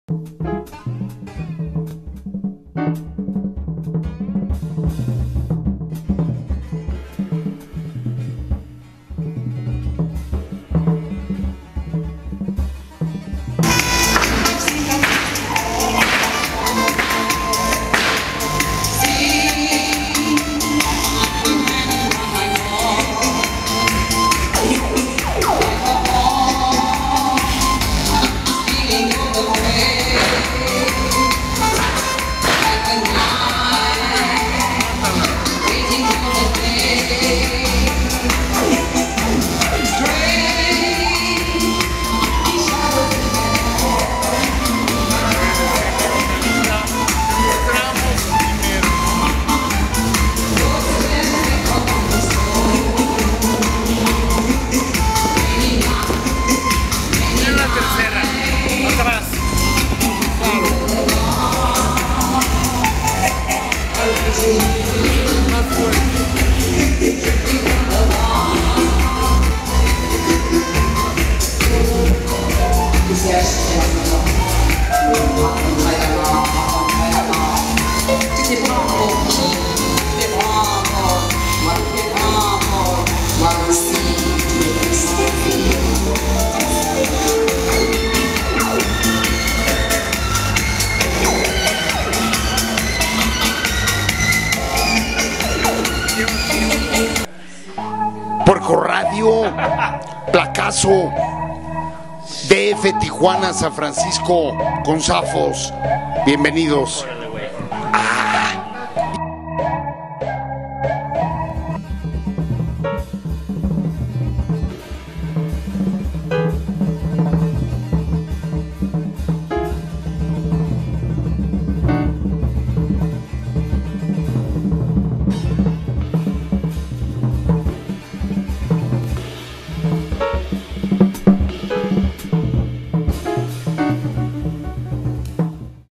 Camarazoo la Pocha Nostra
Un Fragmento de la participación  Guillermo Gómez Peña en esa reunión de acciones denunciantes en el Centro Cultural España, México D.F.